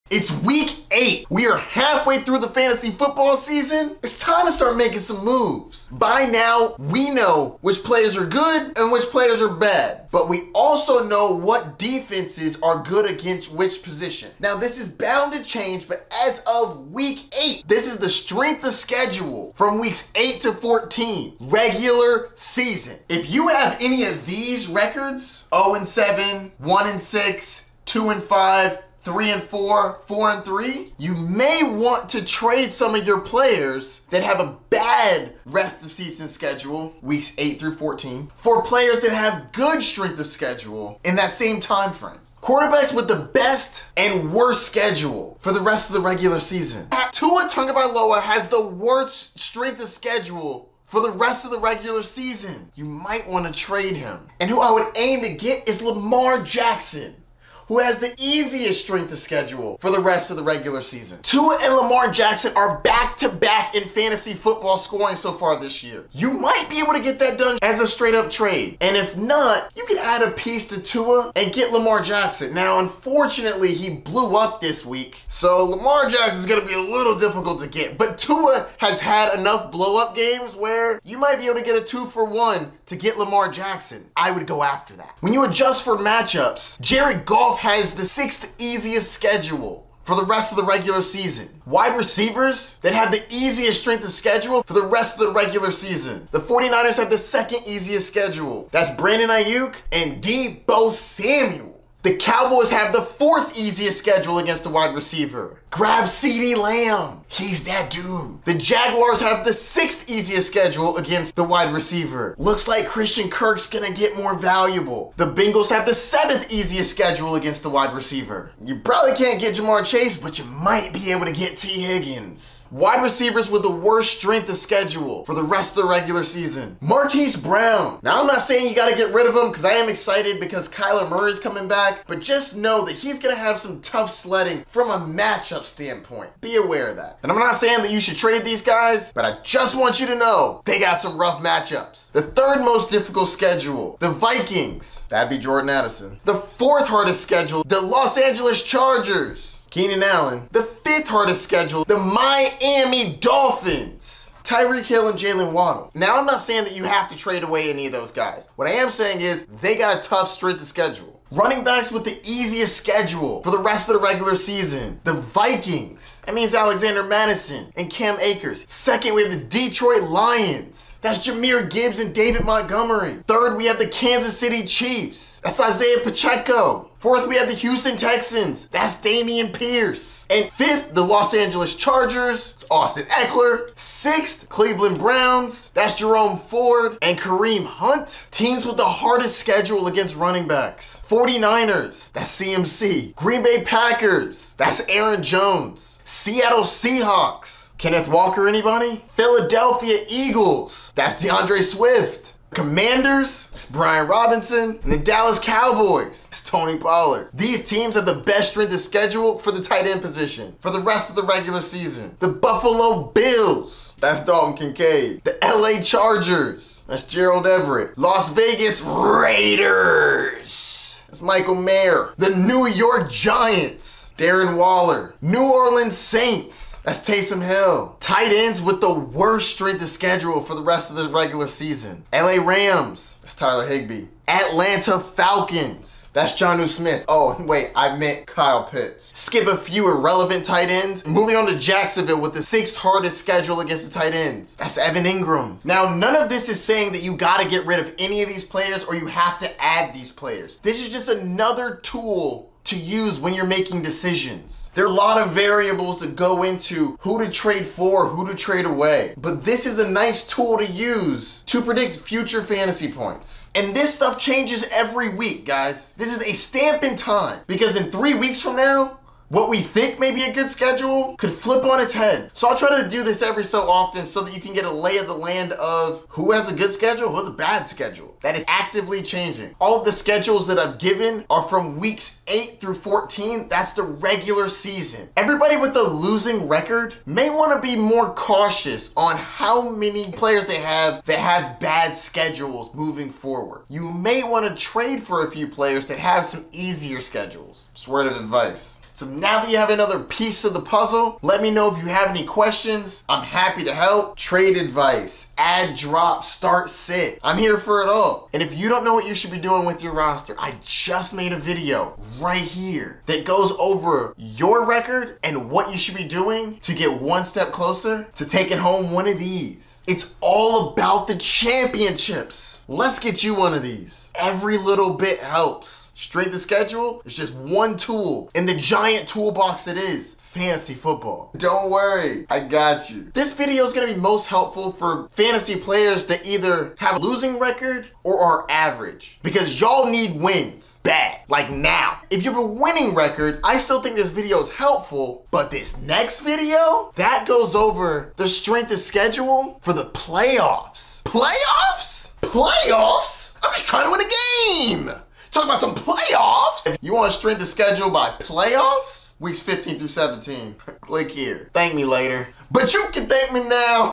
Fantasy Football Strength of Schedule: Your Second Quarter Strategy Guide
In this informative YouTube video, the Fantasy Bay dives into the intricacies of strength of schedule for the second quarter of the fantasy football season. As Week 8 marks the mid-point of the season, viewers are presented with valuable insights into which players and teams have favorable and challenging schedules for Weeks 8 through 14. The video addresses various positions, including quarterbacks, wide receivers, running backs, and tight ends, highlighting players and teams to watch for. It's particularly beneficial for fantasy football enthusiasts looking to make strategic moves, especially if they have a losing record or aim to secure their playoff spots. The narrator stresses that the strength of schedule is just one tool in the vast fantasy football toolbox and provides a link for viewers to explore the strength of schedule for the playoffs. Overall, this video equips viewers with essential information to make data-driven decisions in their fantasy football endeavors.

Time Stamp
0:00 Strength of Schedule
0:42 QB Easy/Hard
1:31 WR Easy Schedule
2:02 WR Hard Schedule
2:41 RB Easy Schedule
3:08 RB Hard Schedule
3:27 TE Easy Schedule
3:47 TE Hard Schedule